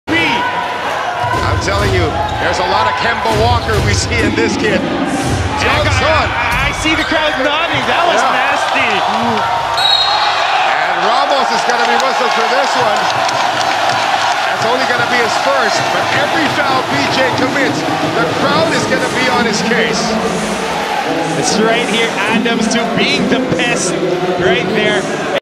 Hong Kong Eastern v San Miguel Alab Pilipinas Ramos hit local HK player then do the new PH trick fal
Ones more Ramos Assault local player this time from HK Wonder how San Miguel the beer band that support this band of basketball